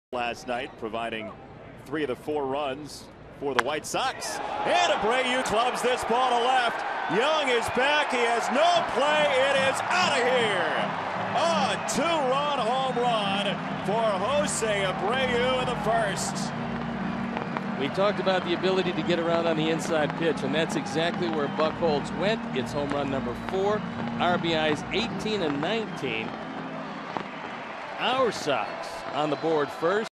BOS@CWS: Abreu jacks a two-run homer in the 1st
Jose Abreu puts a charge into a two-run homer to left field, his fourth of 2016, to open the scoring in the bottom of the 1st inning